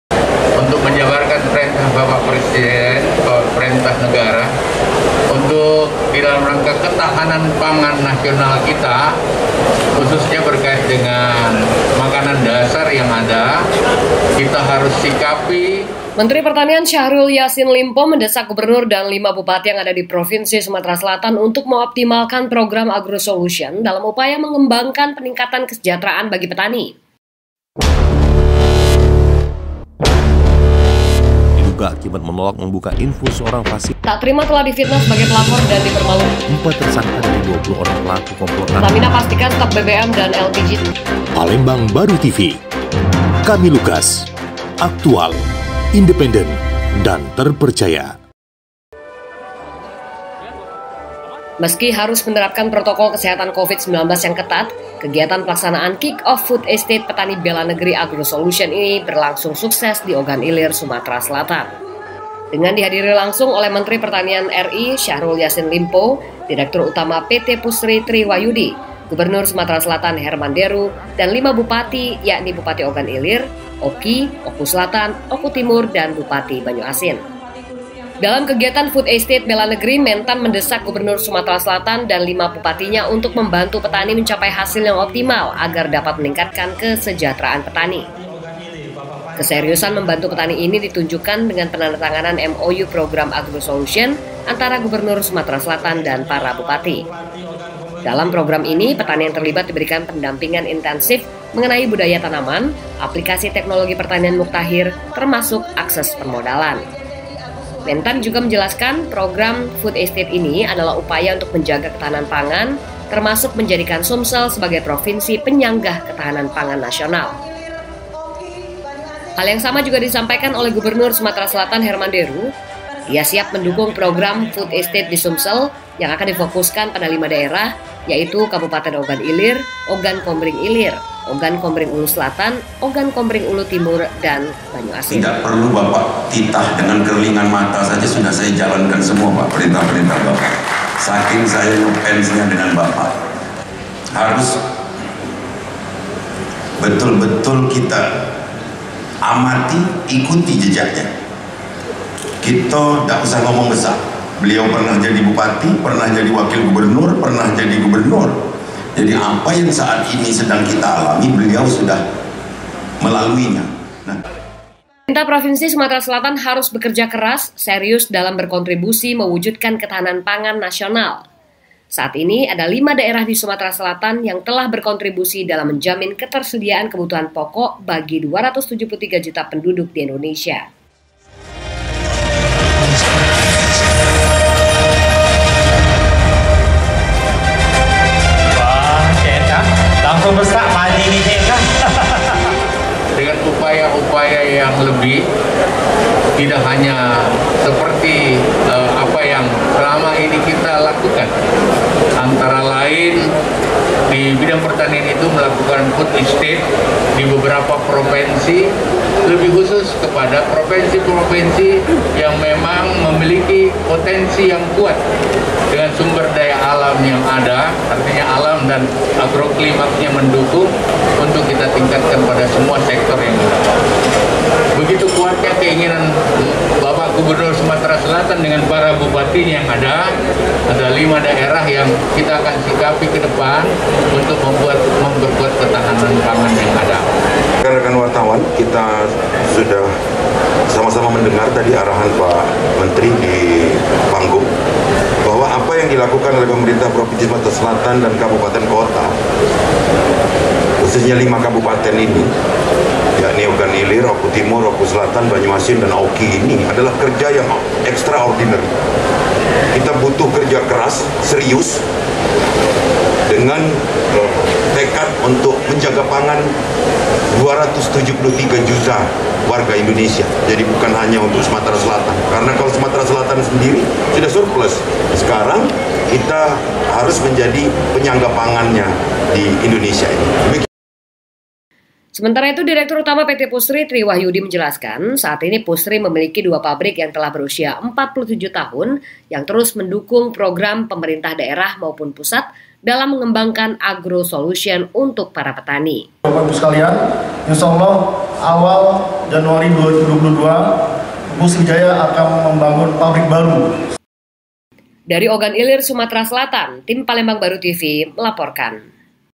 Mentan Ri Desak Gubernur Sumsel dan Lima Bupati Kembangkan Program Agrosolution
Shoot List
WWCR/ Syahrul Yasin Limpo/ Menteri Pertanian RI
WWCR/ Herman Deru/ Gubernur Sumse

Laporan : M Rian